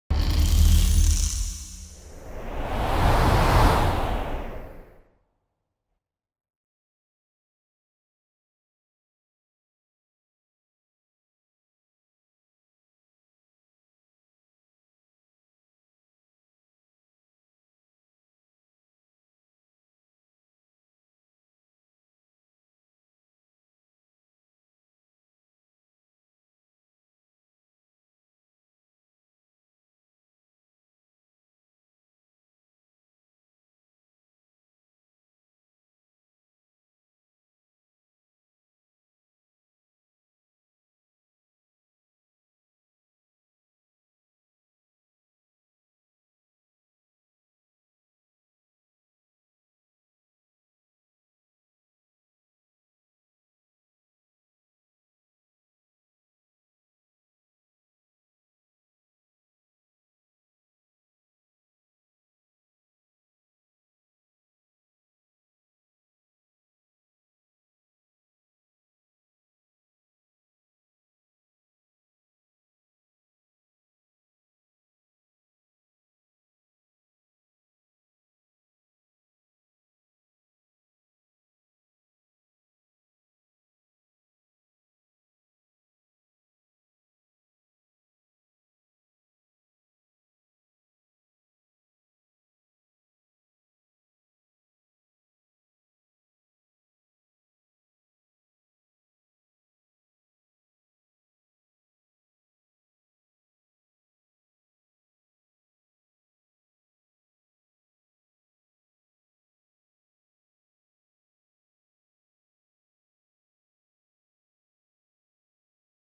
Station terrestre mobile de communication par satellite SYRACUSE-SOTM (SATCOM ON THE MOVE)
Renouveler la capacité SATCOM des véhicules terrestres et en accroître les performances dans le cadre du programme SYRACUSE IV.